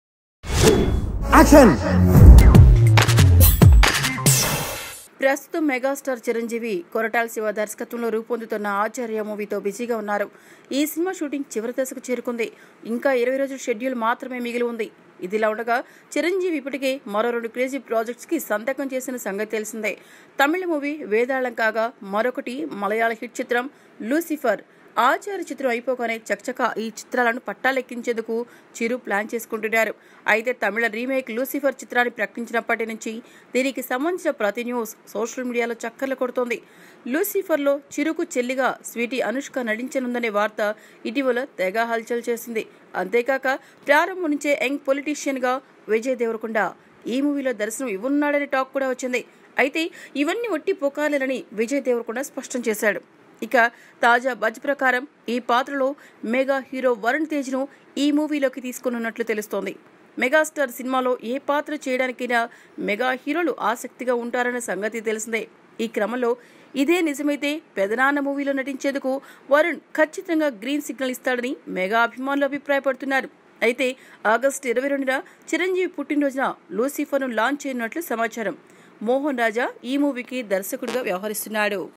Varun Tej To Play Key Role In Chiranjeevi's Lucifer Remake | Friday Poster
Varun Tej To Play Key Role In Chiranjeevi's Lucifer Remake
# Fridayposter Description